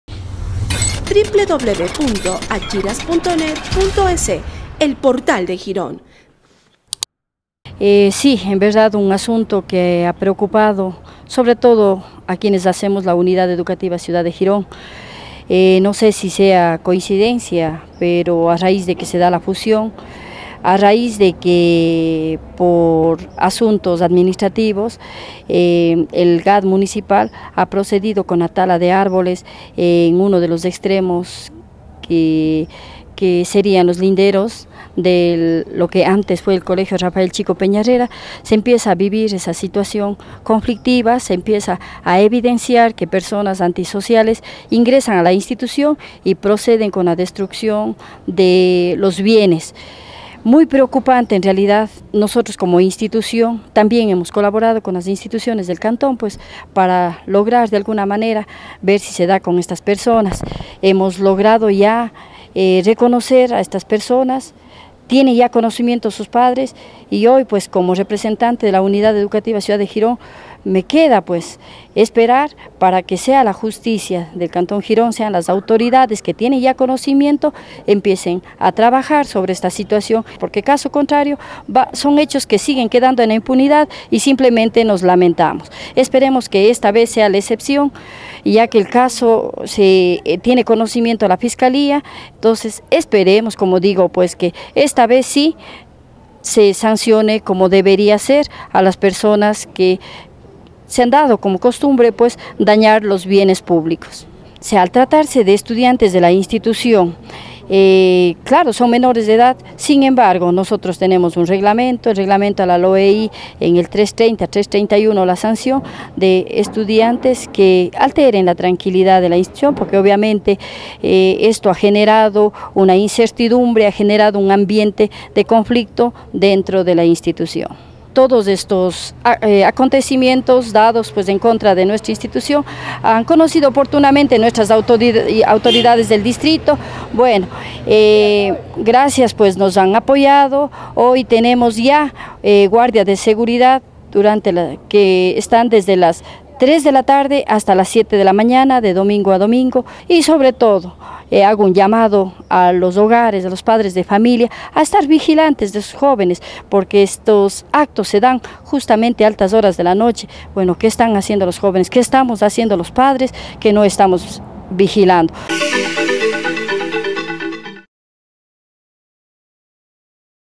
Están identificados quienes destruyeron bienes dentro de colegio
La semana pasada dábamos a conocer sobre hechos vandálicos sucedidos al interior del antes colegio Rafael Chico Peñaherrera y en varias paredes de este centro cantonal, la rectora de la Unidad Educativa Ciudad de Girón, Ing. Eulalia Clavijo, ha manifestado que se tienen identificados a los jóvenes que causaron este desmán por lo que actuarán conforme a lo que dispone el reglamente institucional, a su vez esperan acciones de las autoridades cantonales, se pide también mayor control de los padres hacia sus hijos.